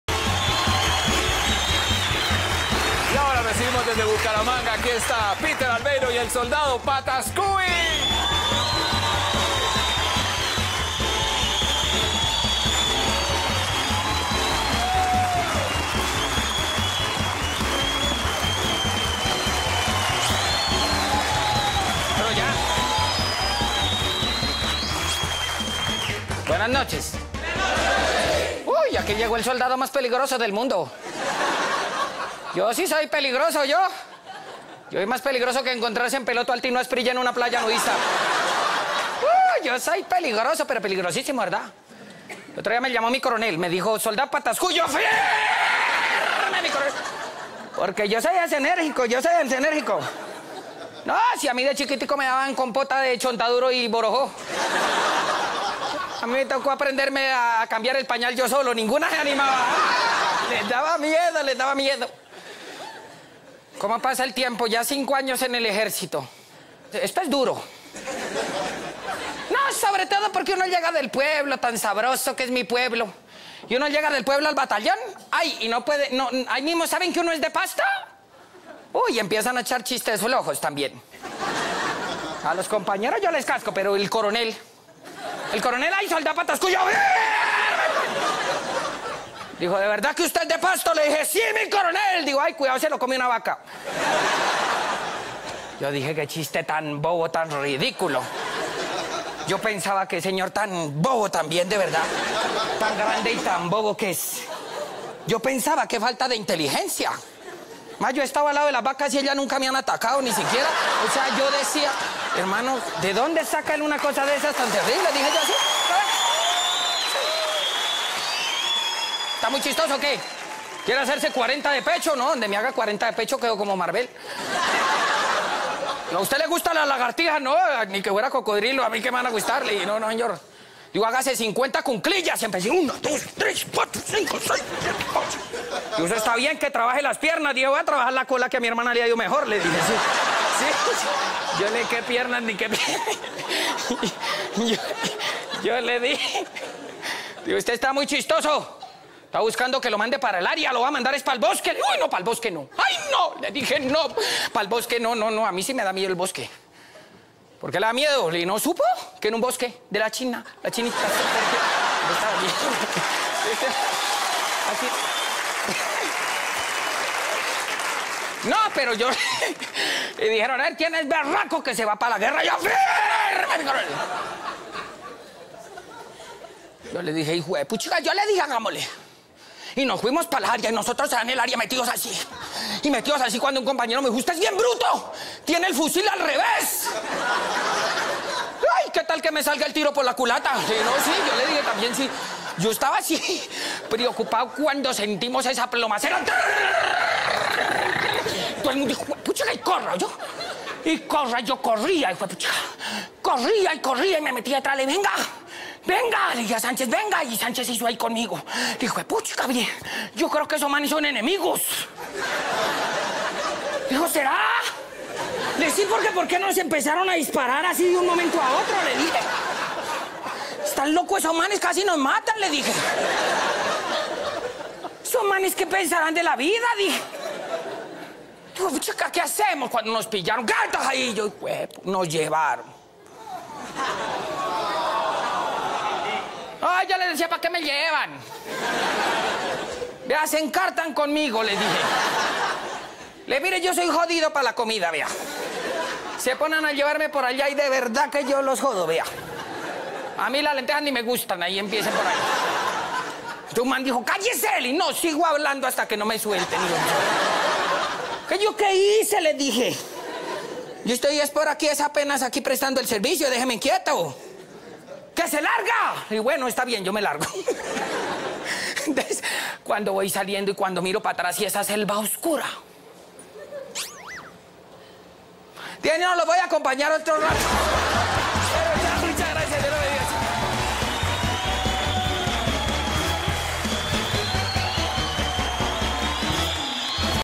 Soy el soldado más peligroso del mundo: Soldado Patascuy
&iexclFirmeeee todo el mundo! Porque los 5 aÃ±os de experiencia en el ejÃ©rcito fueron gracias al entrenamiento que recibiÃ³ de pequeÃ±o, comer compota de chontaduro